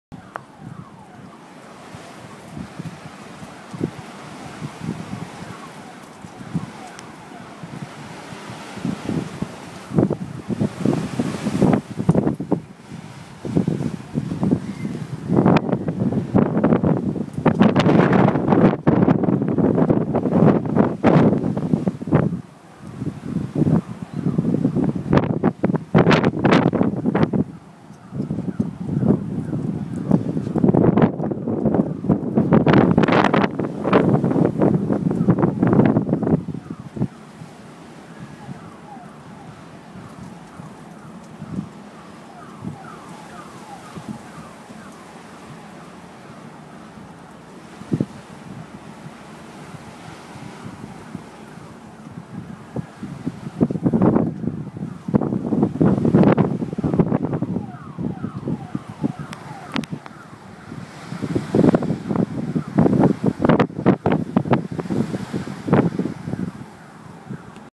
Las Vegas wind storm Jan 21 2012
Some decent winds hit the valley today. My tree is taking a beating.